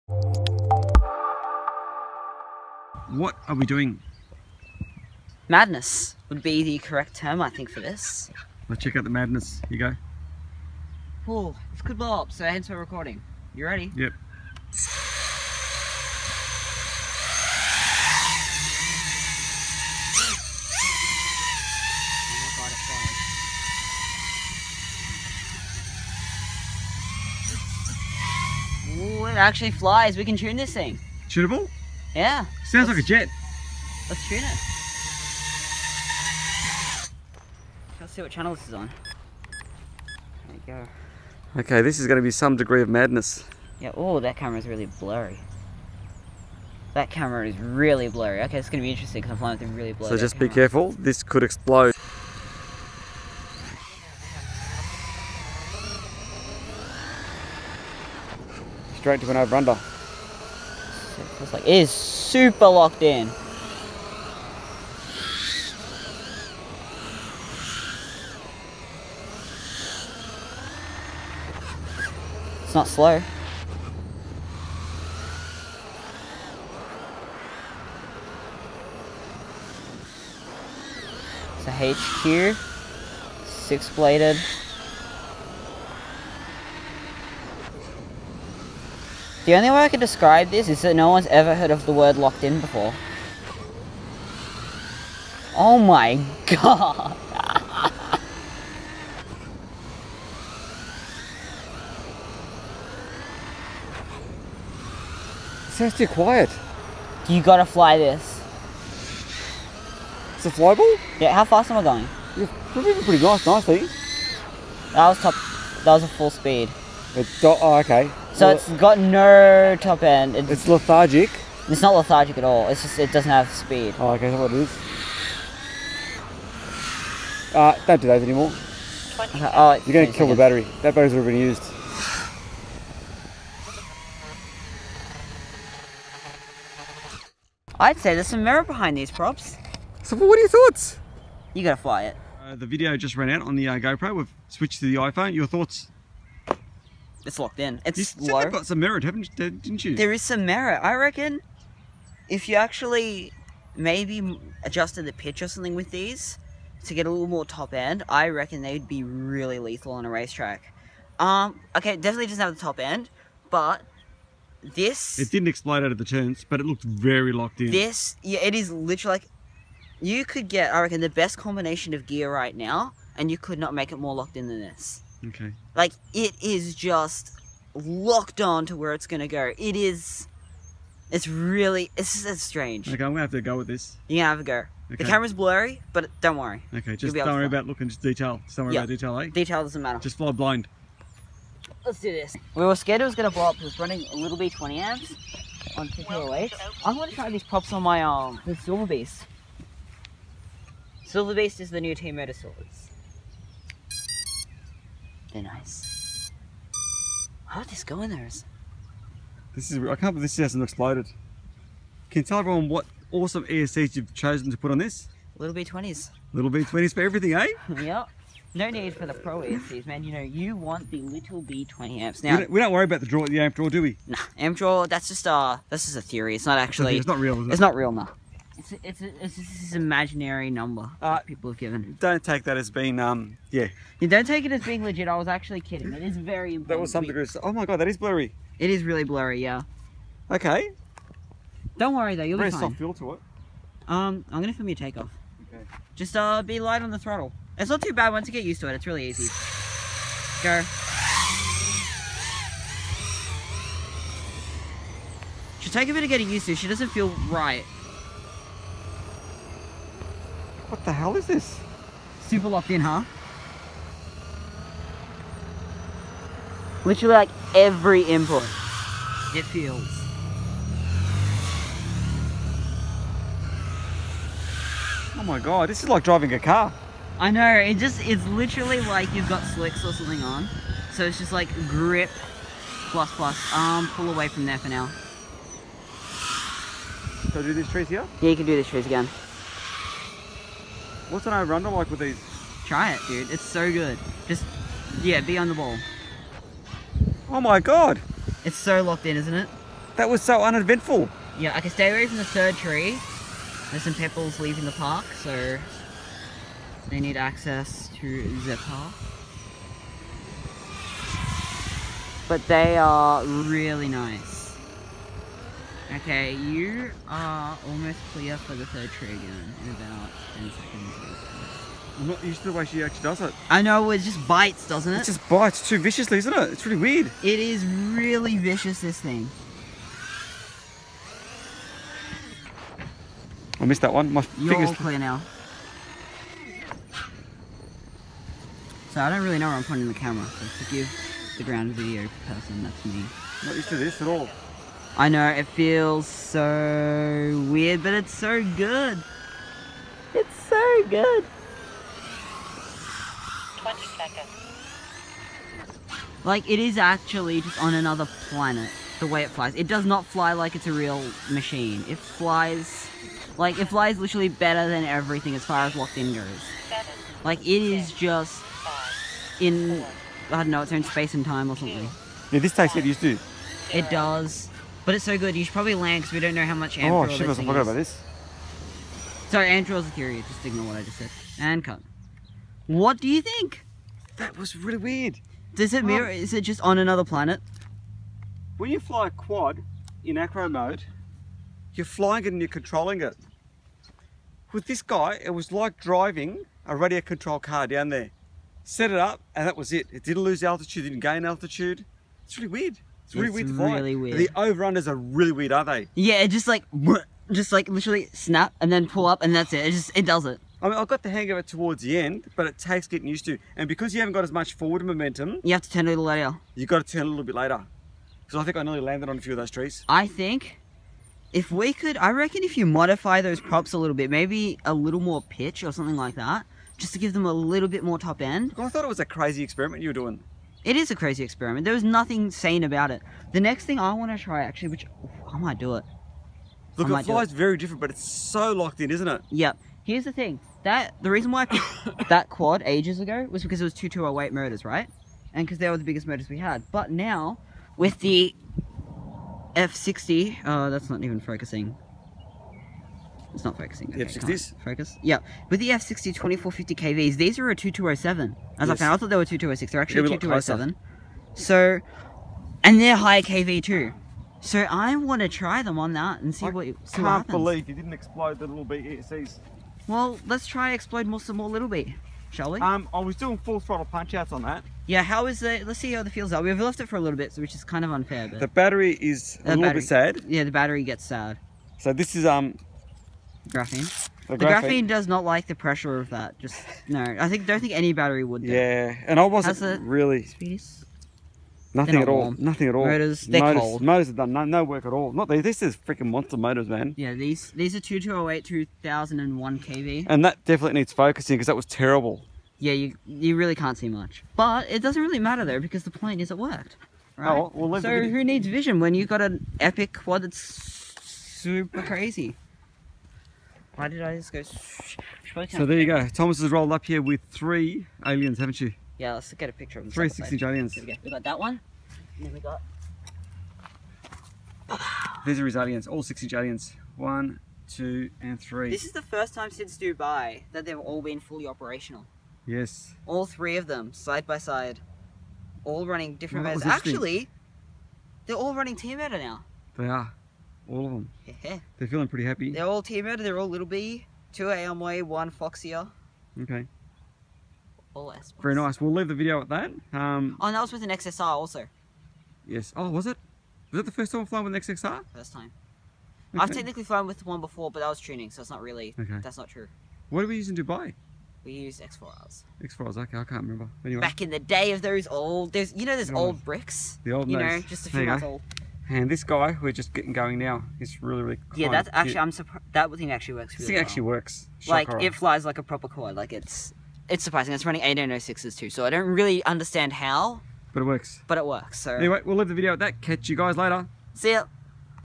6 Blade HQ Prop Madness Flight Test - ImpulseRC Alien
Flight testing 6 Bladed HQ Props!! HQ sent us 6 packs of these props and with all the feedback on them I personally wasn’t going to even bother trying them, however in typical Thomas fashion, he was determined to make these props work. Keep in mind this isn’t an any way meant to be a serious review or anything like that. What you’re seeing here is us testing out the idea and seeing if they were even doable.

We had a set of Monster Volo Edition T-Motor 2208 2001Kv motors pretty much ready to go. They were being used on my 3S Avada prior to being fitted to his 6” Alien.

Thomas had been reading the feedback on these props and correctly decided they needed way more power to drive these motors. Most people had been testing them on 2205 motors which just didn’t have the grunt needed to turn so many blades.

So keep in mind this is Paul writing this and this is my opinion, I doubt Thomas would agree with me even though I’m sure he knows I’m right lol The 6 Bladed props were very locked in and down low they seemed responsive when using such a big motor. Like Thomas Said in the video they lacked top end in a major way, but that wasn’t the biggest problem. The real problem was how much they were hurting the battery, so running these props with the right motor would mean a serious reduction in battery life.

Now having said all that, boy was it fun to fly! So that being said this obviously wasn’t meant to be a serious video, more just Thomas and I having a bit of fun with his crazy ideas.

Basic Quad overview
ImpulseRC 6” Alien
HQ 5x4x6 Blades
Litte Bee 20amp ESC (Should use higher rated ESCs than these!)
MotoLab Cyclone FC
Beta Flight 2.2.0
HS1177 FPV Camera
T-Motor Volo Edition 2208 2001v